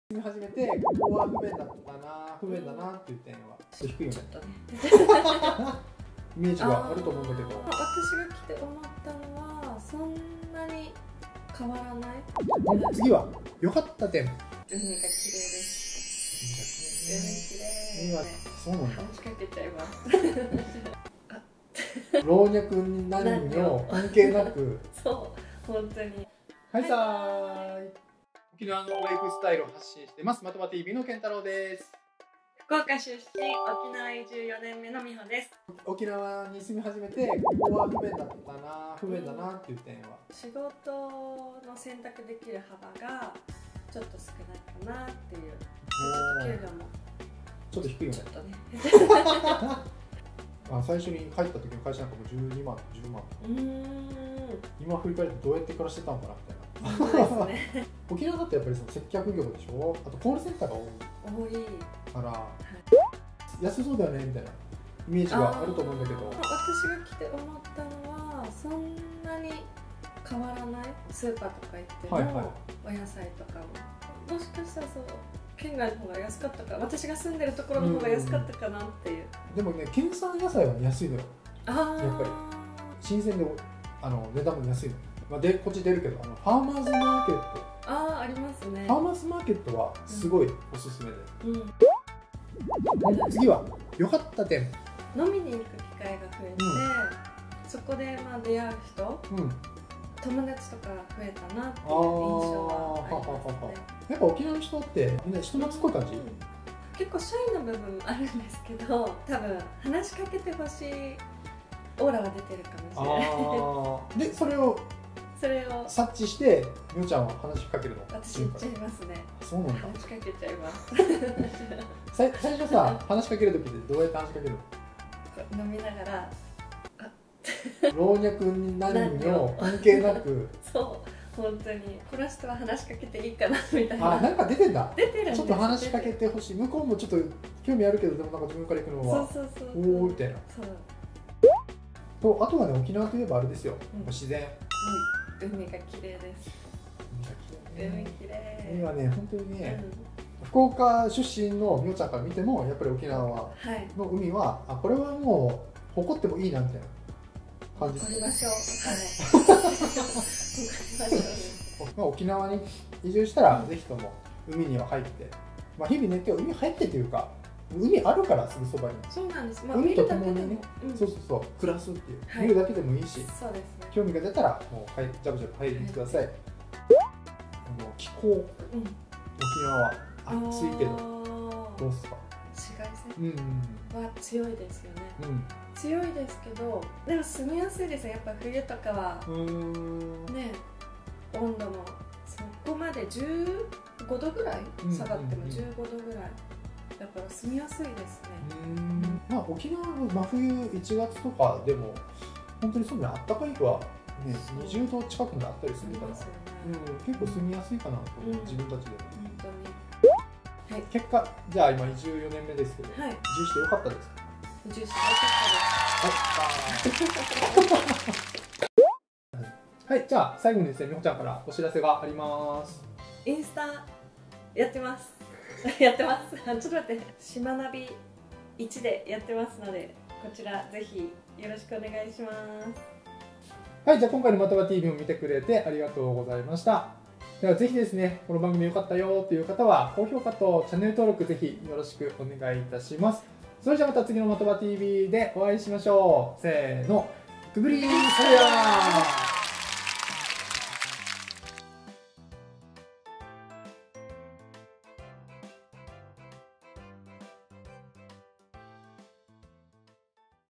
【沖縄移住】福岡出身女子が選ぶ！ 沖縄に住んでよかったコト・不便なコト【沖縄旅行気分】
「沖縄好きが増えたらいいな」を合言葉に、沖縄の観光スポットや沖縄ライフスタイルを発信中！

今回は、福岡出身のミホちゃんが沖縄に移住した際によかったコト、不便だったコトを聞いてみました。

・沖縄はやっぱり給料が低め…!?
・物価は実は高い方!?
・沖縄移住＝海とともに暮らすこと!?